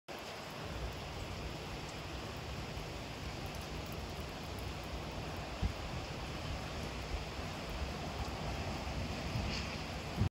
Backyard Visitors
These two show up each year and have even laid eggs in the garden before.